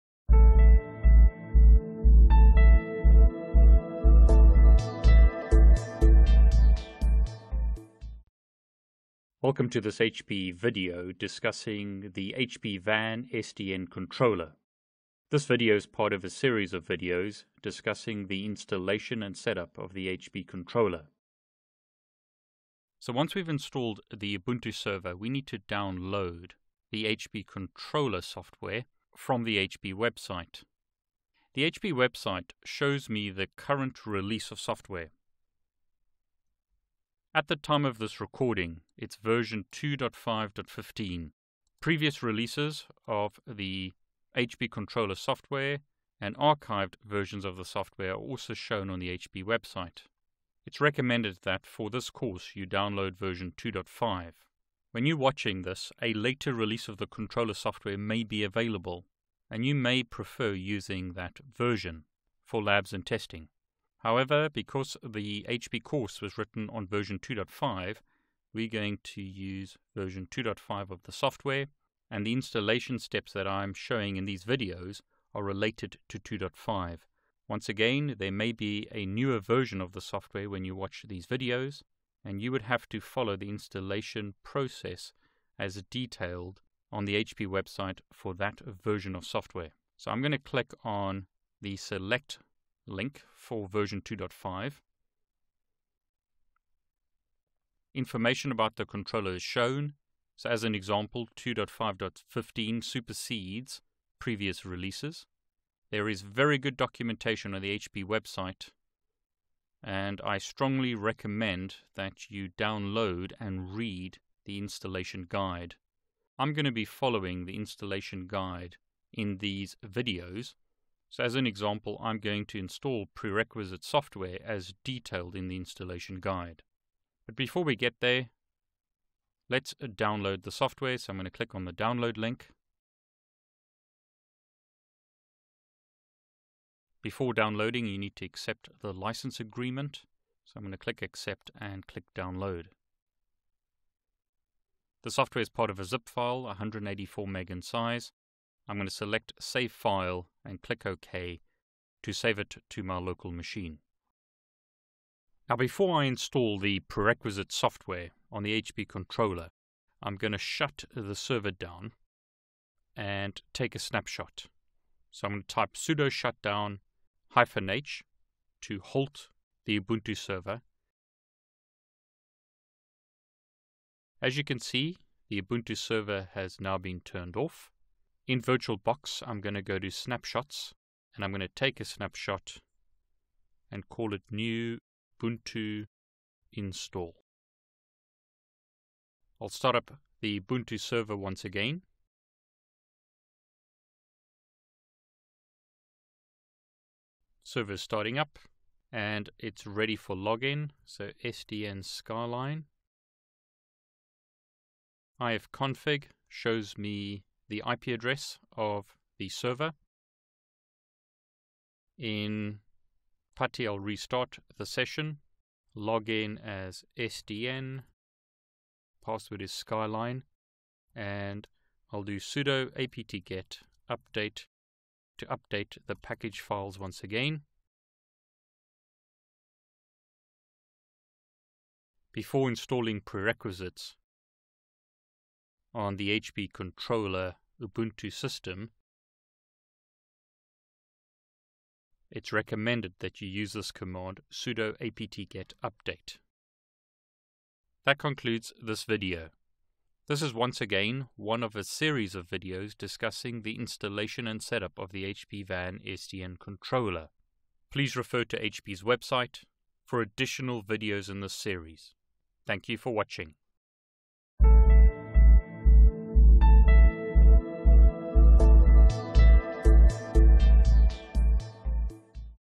HPE VAN SDN Controller Installation-HP Controller Download
Fourth video in a series of 7 videos on how to install the HPE VAN SDN Controller